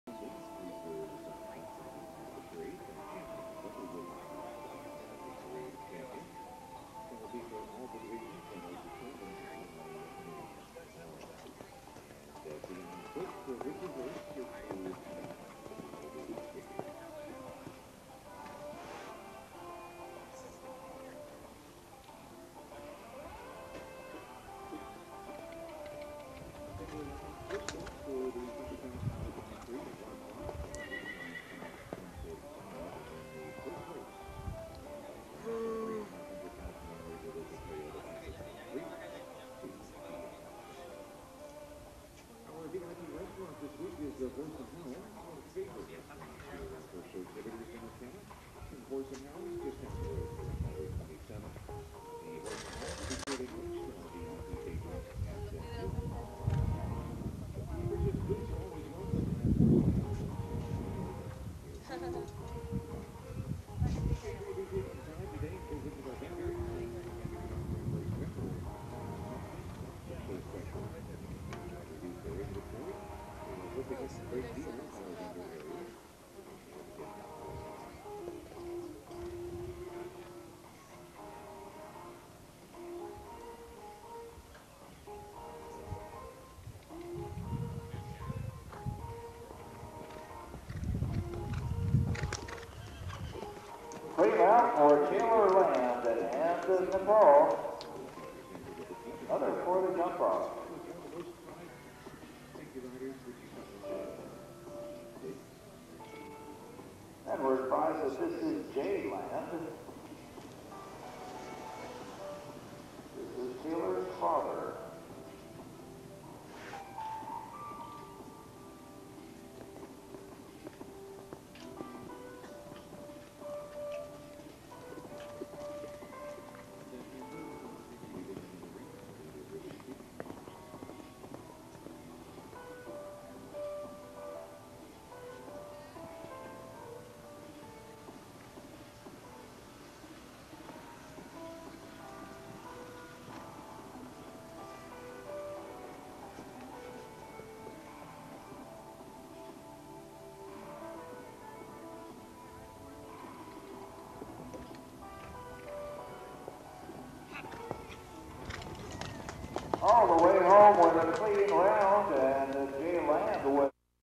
Nepal Ocala2 09
Nepal with Jay Land in the $15,000 Junior/AO Classic in Ocala 2009.